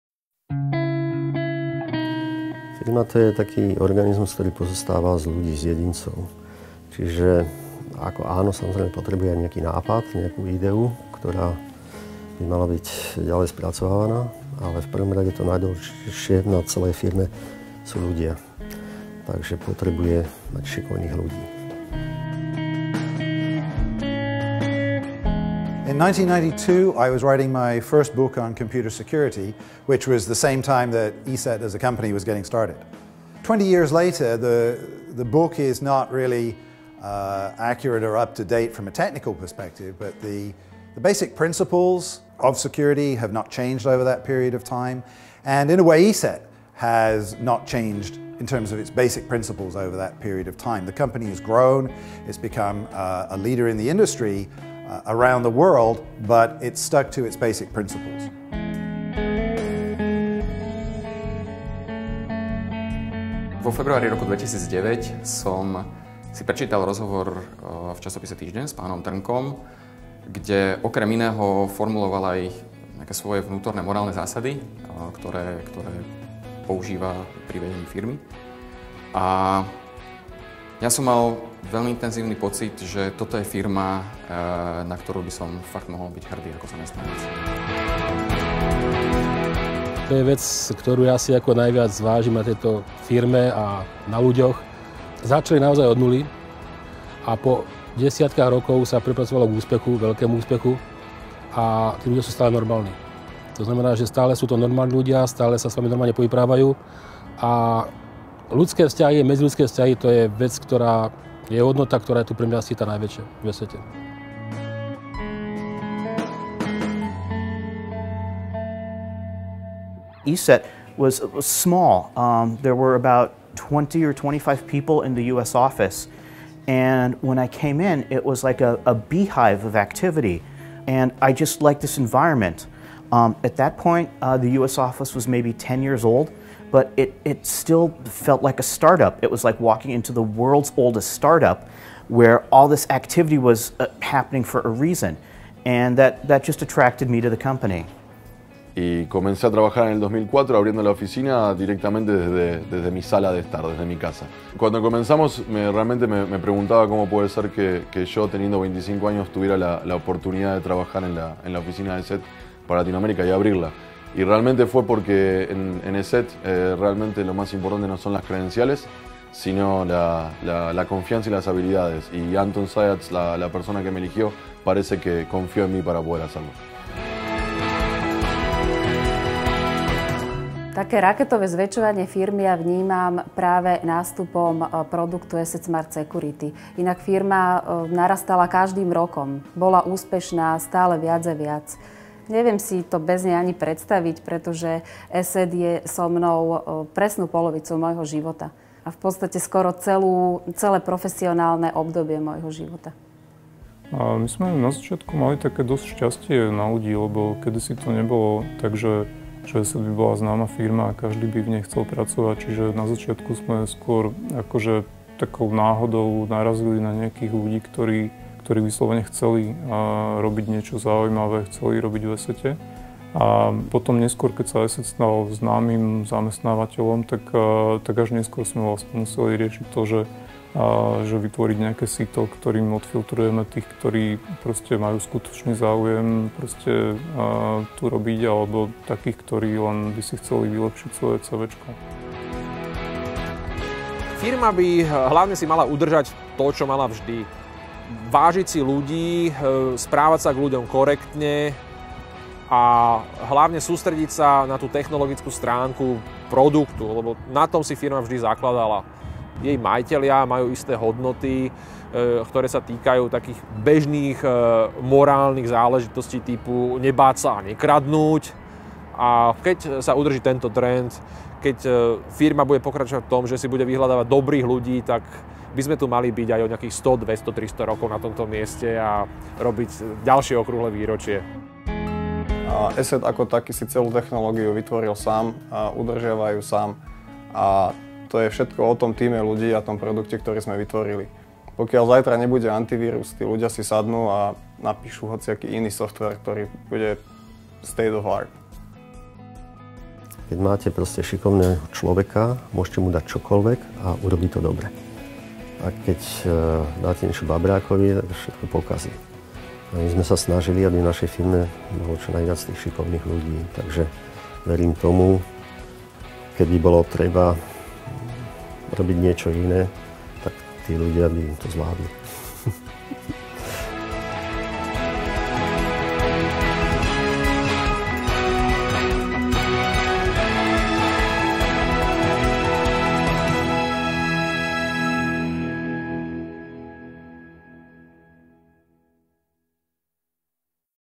25 years of ESET, its technology and passion for security
ESET, the leader in proactive digital protection - developing award-winning technology for more than 25 years is a company that has grown up on human enthusiasm, values and determination to protect users.